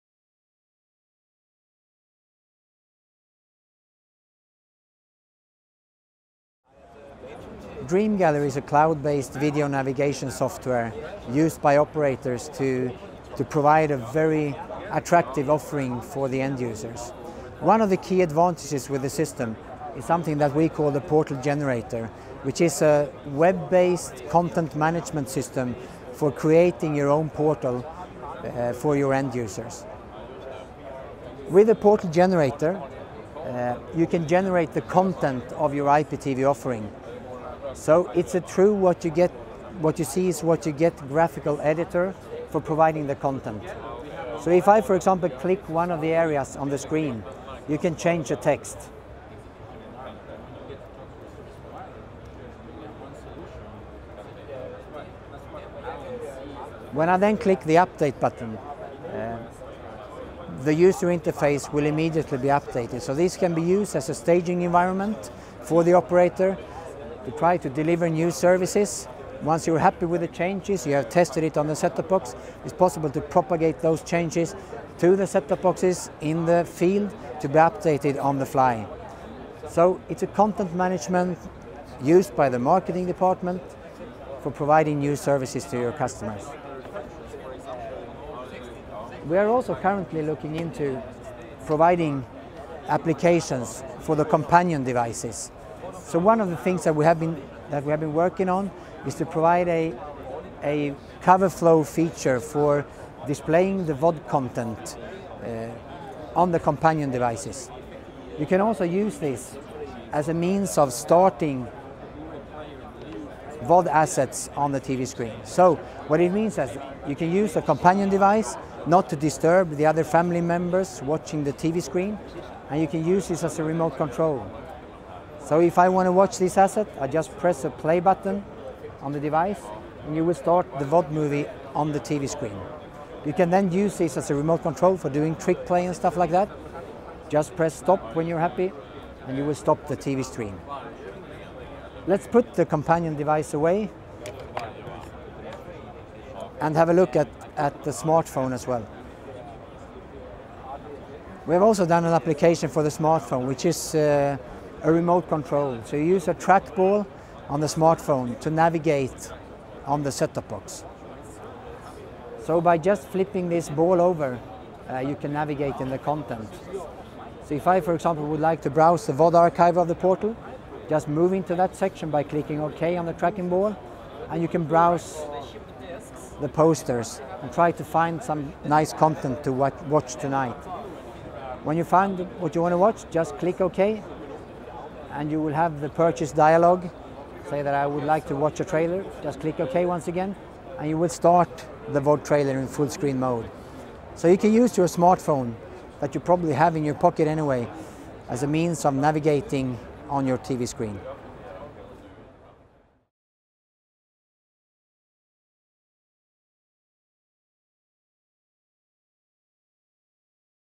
IBC 2011: Dreamgallery multi-screen UI
Daniel Karlsson, Motorola Mobility outlines how Dreamgallery gives operators complete customization control of their television portals for advanced user navigation, features, and functionality for the deployment of new video experiences to the set top and beyond.